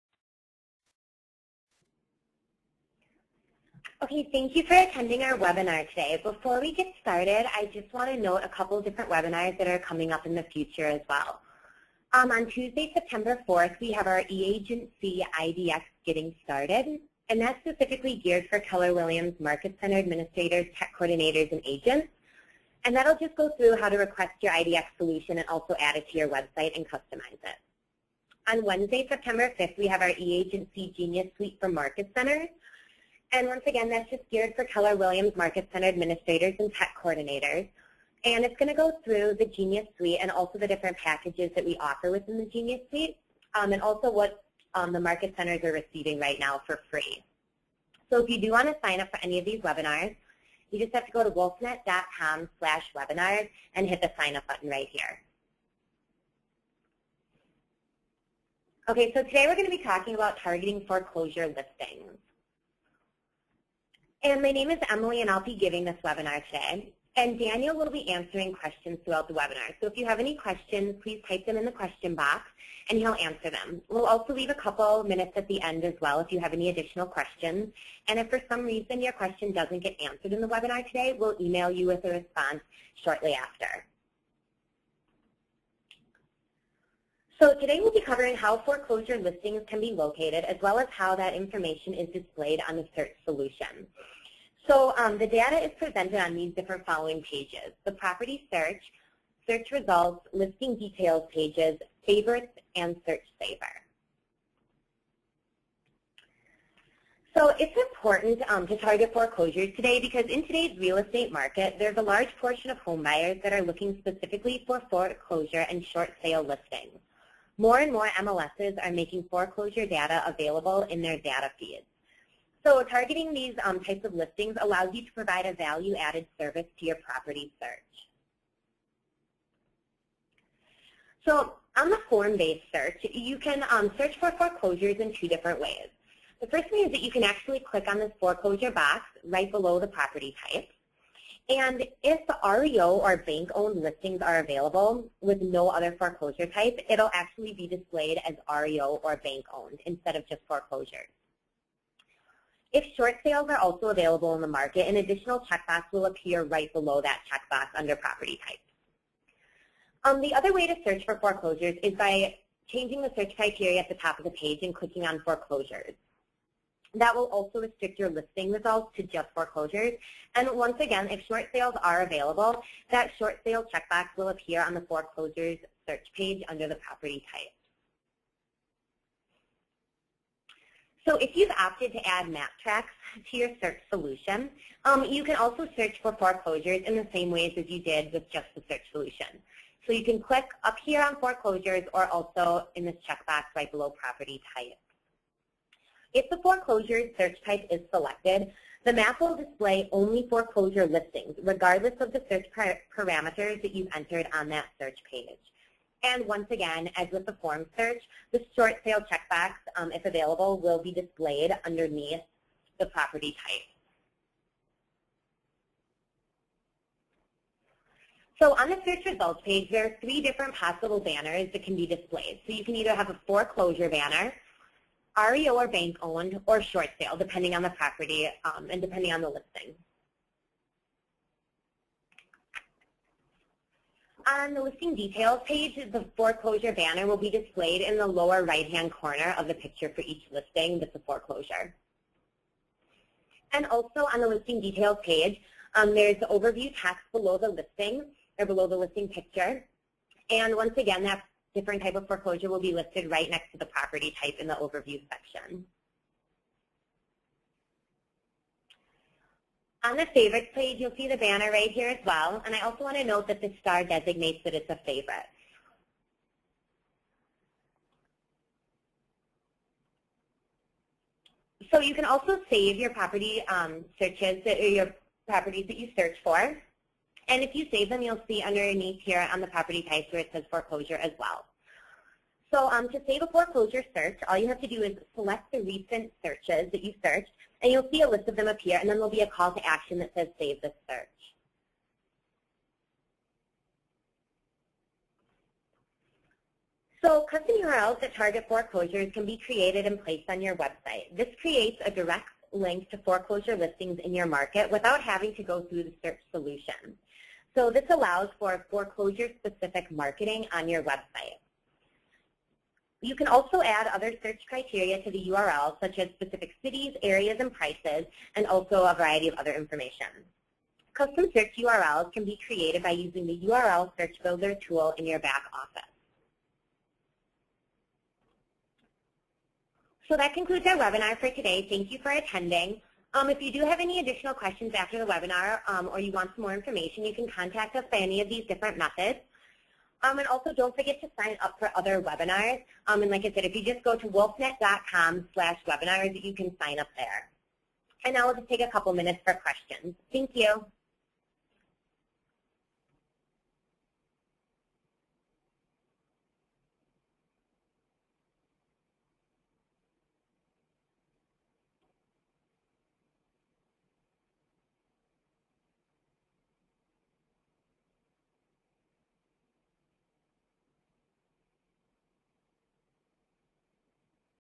Targeting Foreclosure Listings
Join us to learn how to customize your property search to target and highlight Foreclosure, Bank Owned/REO and Short Sale listings!

Audience: All new and existing customers